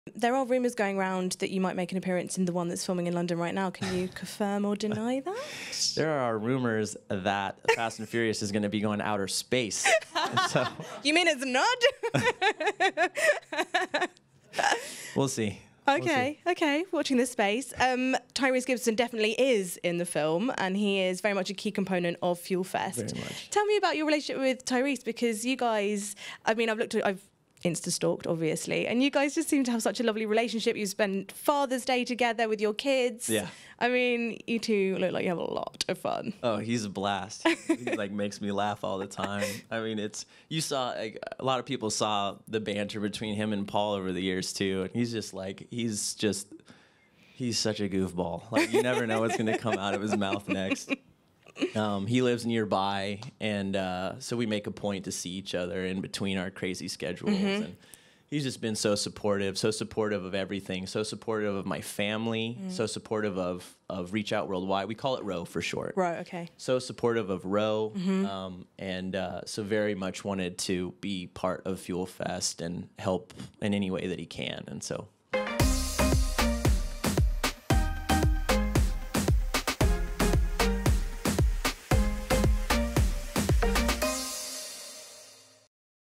Cody Walker & His Relationship with Tyrese Gibson
Furious 7 star Cody Walker opens up to BUILD about his relationship with Fast and Furious royalty, Tyrese Gibson. Cody talks about how the two stars have become great friends, as well as how Tyrese is the biggest ‘goofball’ and that they even spent Fathers Day together with their children.